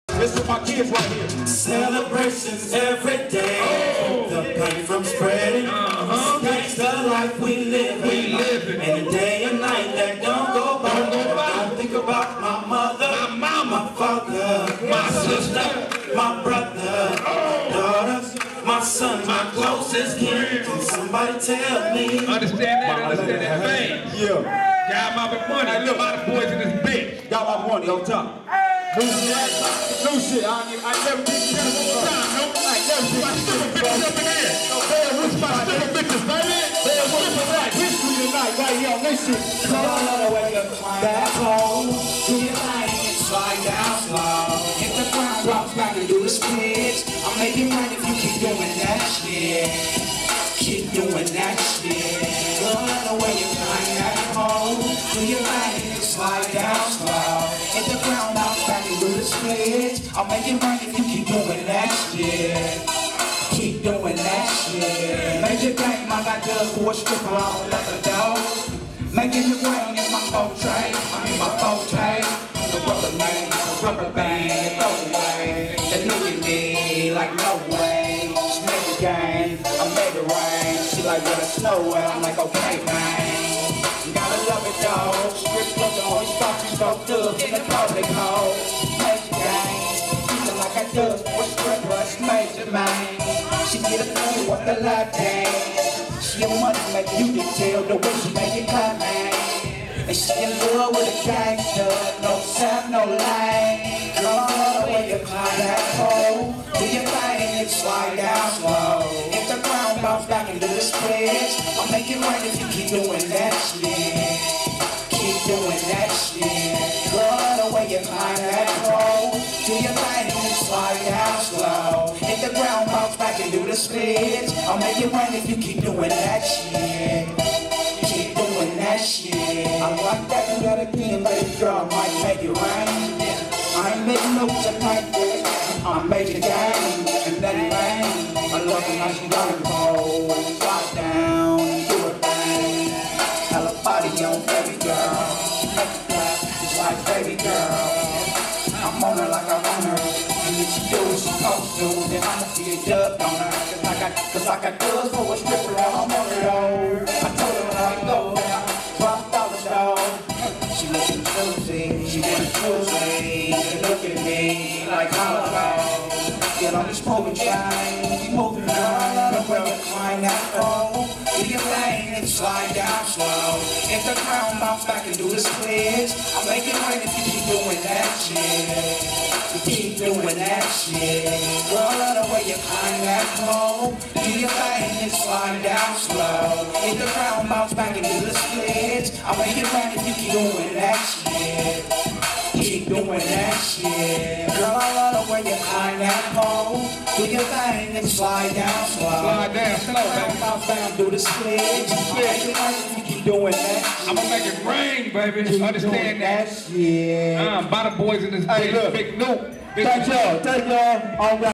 THEREALBIGNUKE @ SHOTCALLERZ
PJIZLE FILMZ CAPTURE BIG NUKE AT THE ALL WHITE PARTY IN THE HIGH DESERT